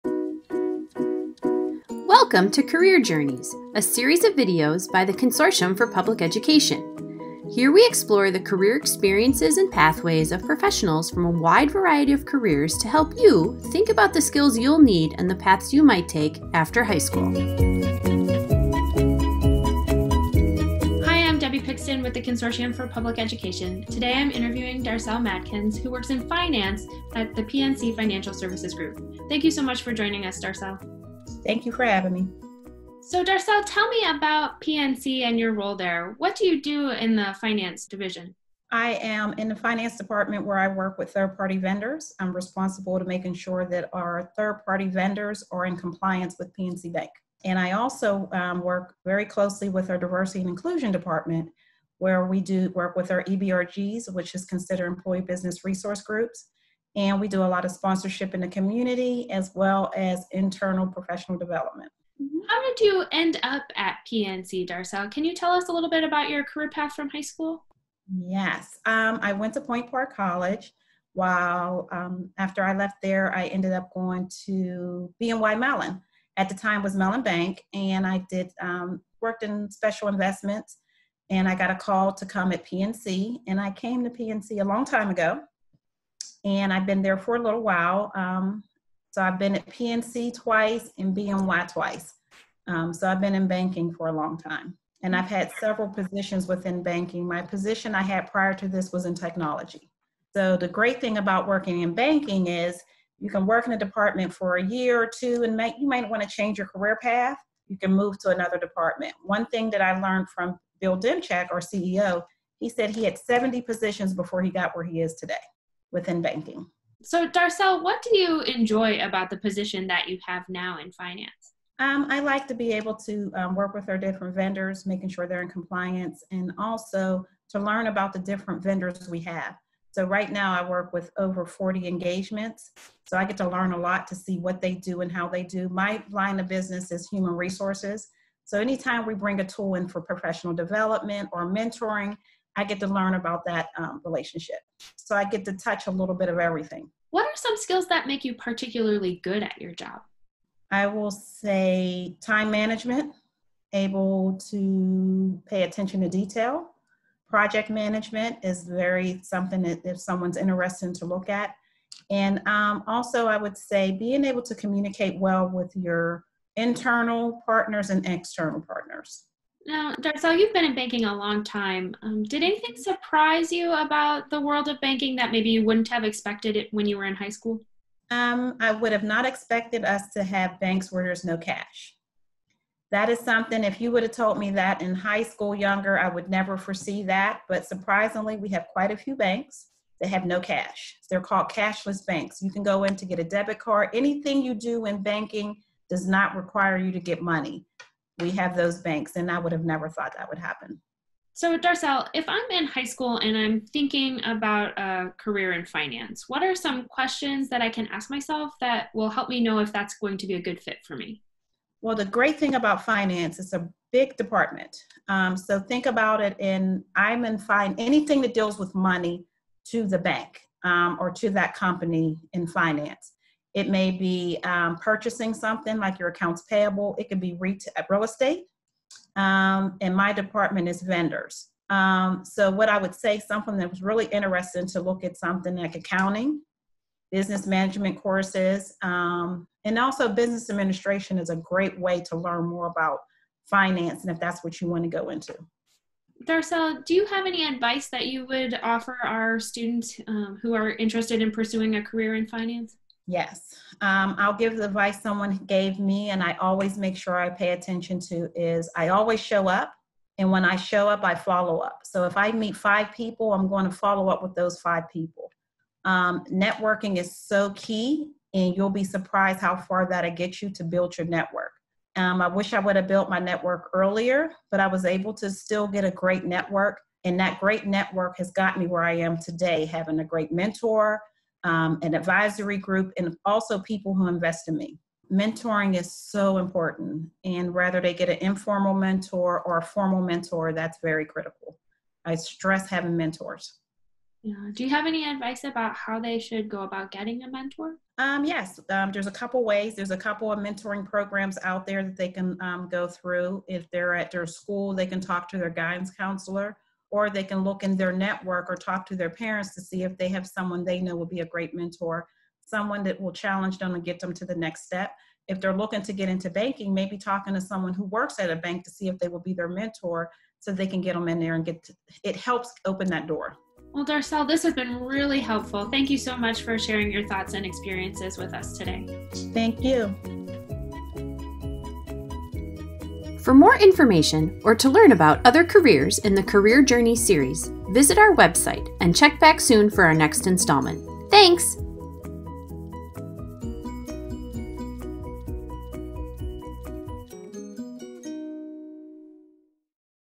Career Journeys with Darcel Madkins in Finance
Hear about Darcel Madkins' journey to becoming an Enterprise Third Party Management and Vendor Relationship Risk Specialist at PNC Financial Services in this Career Journeys interview.

Career Journeys is a series of video interviews with professionals from across all sixteen Department of Labor career clusters to help students gain insights into possible careers.